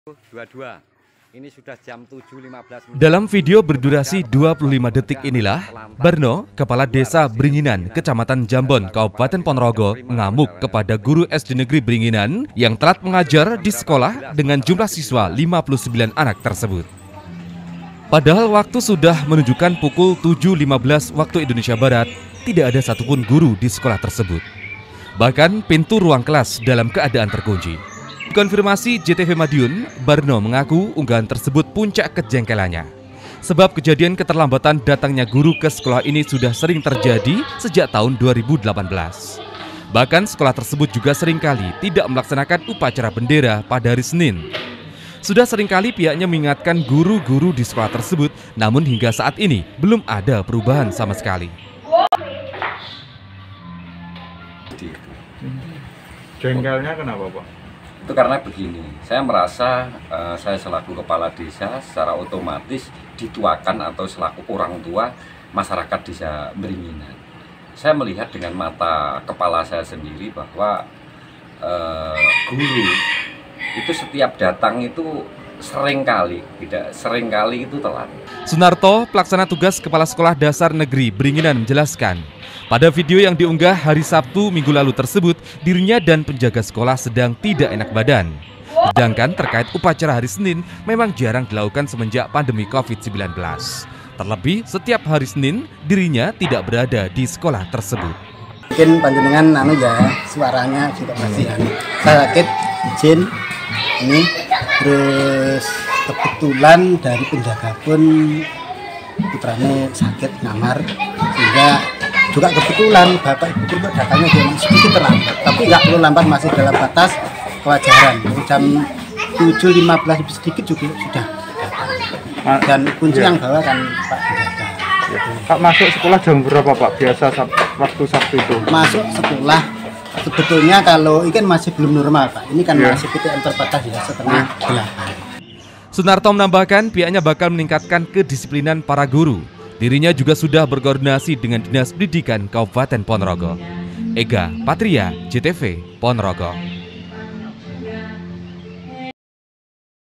PONOROGO - Puluhan Siswa Terlantar Gegara Guru Terlambat, Kades Bringinan Ngamuk Di MEDSOS
Diduga guru SDN Bringinan telat datang ke sekolah menyebabkan siswa terlantar. Kades menyebut perilaku tak patut dicontoh itu sejak terjadi sejak tahun 2018.